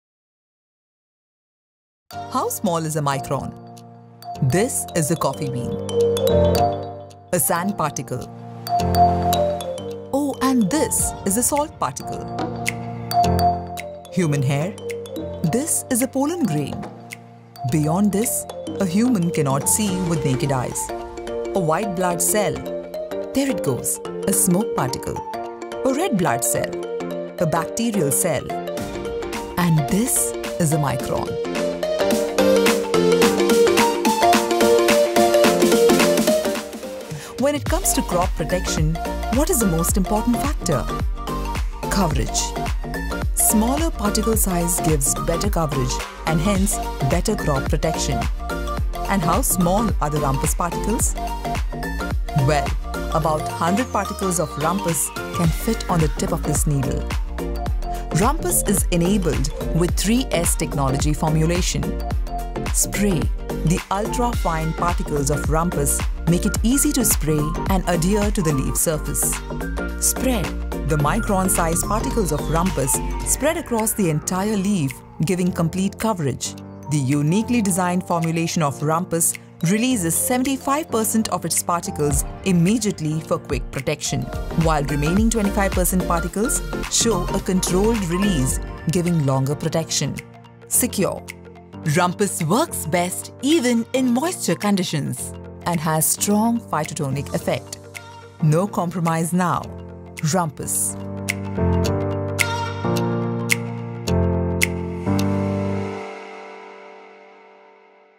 RUMPUS No Compromise Now...The power of micron-size particles. New Fungicide by Sumitomo Chemical
How small is a micron?
This is a coffee bean
A sand particle
Oh, and this is a salt particle
Human hair
This is a pollen grain
Beyond this, a human cannot see with naked eyes
A white blood cell (shown on petriplate under a microscope)
There it goes…a smoke particle
A red blood cell
A bacterial cell
And this ….is a micron
When it comes to crop protection, what is the most important factor?
Coverage
Smaller particle size gives better coverage and hence better protection
And how small are the Rumpus particles?
Well, about 100 particles of Rumpus can fit on the tip of this needle
Rumpus is enabled with 3S technology formulation
Spray- The ultra-fine particles of Rumpus make it easy to spray and adhere to the leaf surface.
Spread- The micron size particles of Rumpus spread across the entire leaf giving complete coverage. The uniquely designed formulation of Rumpus releases 75% of its particles immediately for quick protection, while remaining 25% particles show a controlled release giving longer protection.
Secure- Rumpus works best in moisture conditions and has strong phytotonic effect.
No Compromise Now!
Rumpus